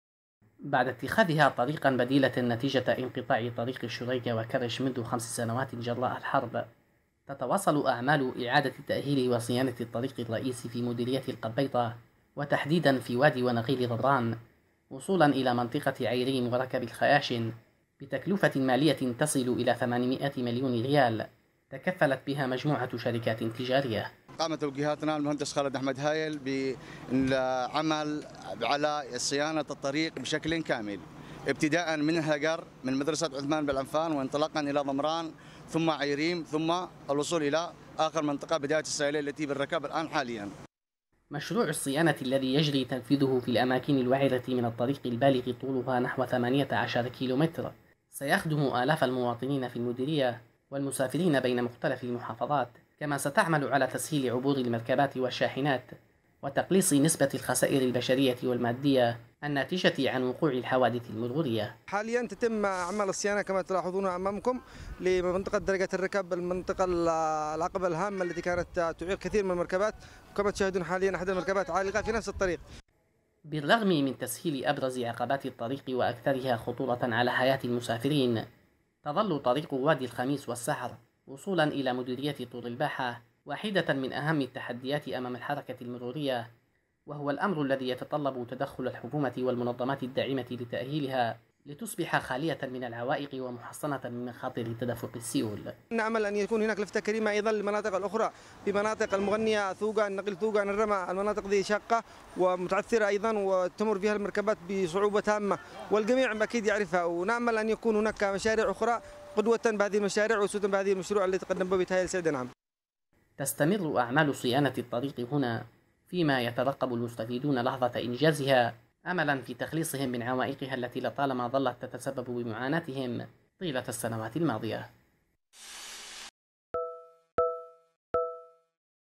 لحج.. إعادة تأهيل طريق القبيطة الرابطة بين تعز وعدن بعد خمس سنوات من انقطاعها| تقرير: سامح عبدالوهاب
تجري في مديرية القبيطة بمحافظة لحج أعمال صيانةِ وتأهيلِ الطريق الرئيسة الرابطة بين تعز وعدن من جهة كرش والراهدة.
وقالت الجهات المنفذة للمشروع، إن تكلفة إعادة تأهيل الطريق تصل إلى ثمانِمائة مليون ريال، وذلك بعد نحو خمس سنوات من انقطاعها.
ويعاني المسافرون وسائقو الشاحنات من وعورة الطرق التي تربط عدن وتعز جراء الحرب، والسيول التي دمرت معظمها...